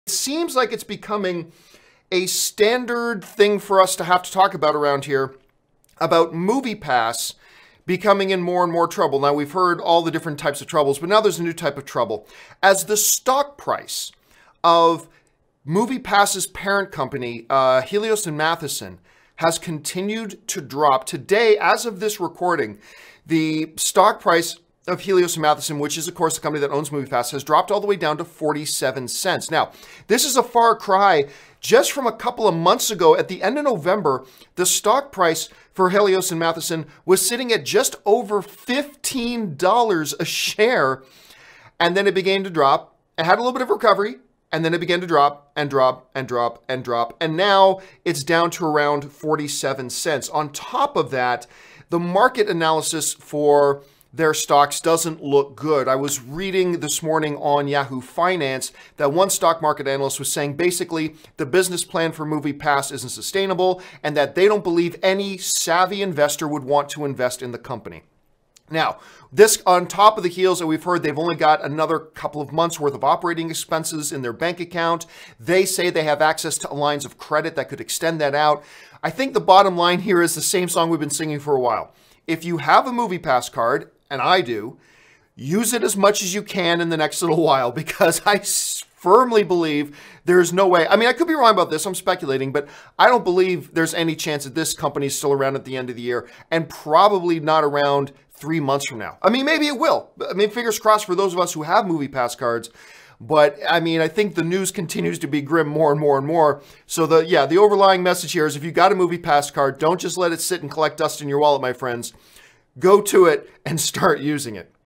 Moviepass Stock Continues To Fall - Use It While You Can
Moviepass continues to struggle as the stock value of their parent company continues to fall. The $10 per month model isn't working and now members need to use their service as much as they can now before the company goes under